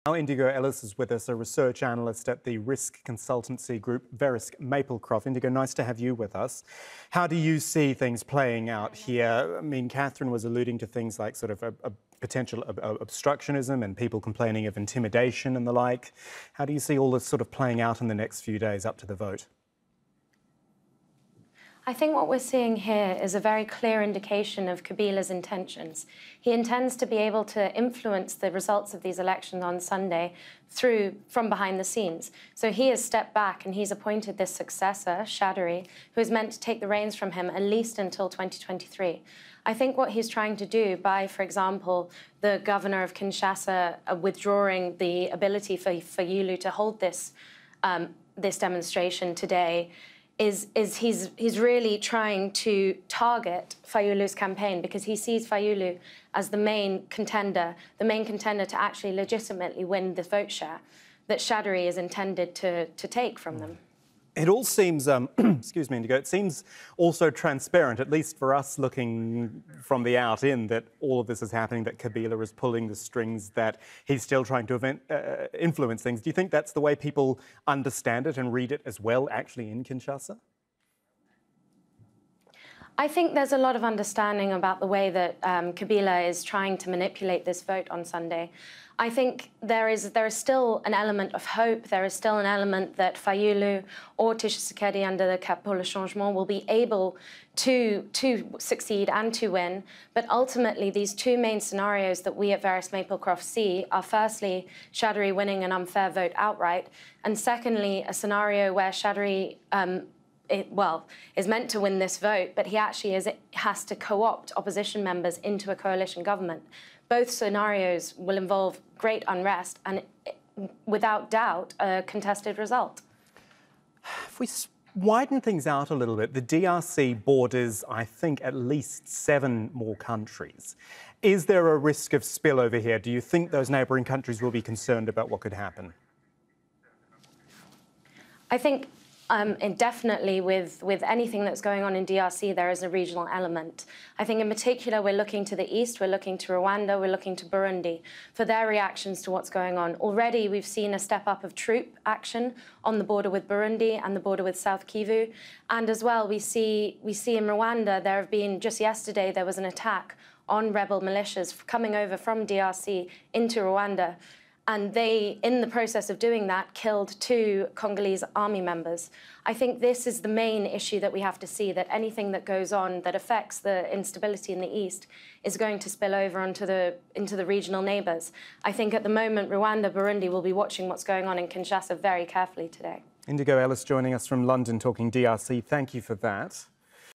🇨🇩DRC election: Governor bans campaigning in capital Kinshasa | Al Jazeera English
The governor of Kinshasa, capital of the Democratic Republic of the Congo, has banned campaigning in the city ahead of Sunday's presidential vote for security reasons, a move denounced by the opposition.

Andre Kimbuta, a member of the ruling coalition, announced his decision on Wednesday following crackdowns by security forces on opposition supporters last week that killed at least seven people and caused a fire that destroyed thousands of voting machines.

"The information in our possession attests that in all the political camps of the main presidential candidates, extremists have prepared and are preparing for a street confrontation in the city of Kinshasa during election campaign activities," Kimbuta said in a statement, without elaborating.

The long-delayed election is meant to choose a successor for President Joseph Kabila, who is due to step down after 18 years in power. If successful, it would be the DRC's first democratic transfer of power.

Kinshasa is an opposition stronghold, where security forces killed dozens of people during protests against Kabila's refusal to step down when his mandate officially expired two years ago.

Indigo Ellis, a research analyst at the risk consultancy Verisk Maplecroft, talks to Al Jazeera about the risks of political turmoil in the country, which could spill over across the border.